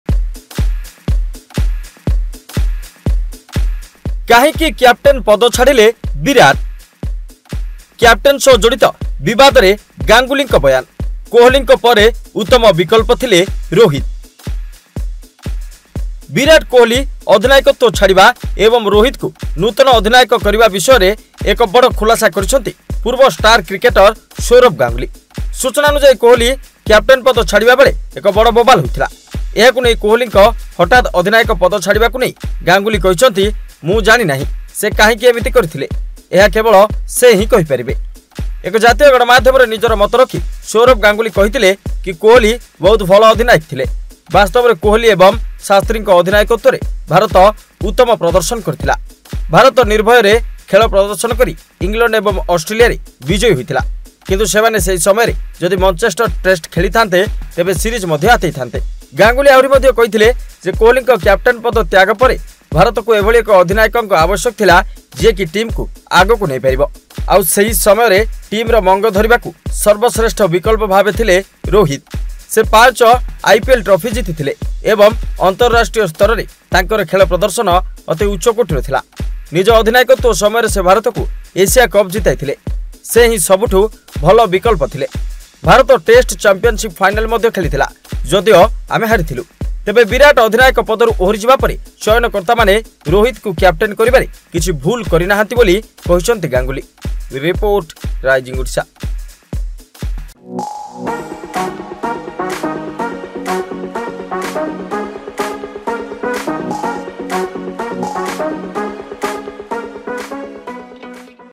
Why Virat Kohli Left Captaincy?; Ex Team India Captain Sourav Ganguly's Surpirising Statement | News
Sourav Ganguly Makes A Shocking Claim on Virat Kohli's Test Captaincy: Sourav Ganguly VS Virat Kohli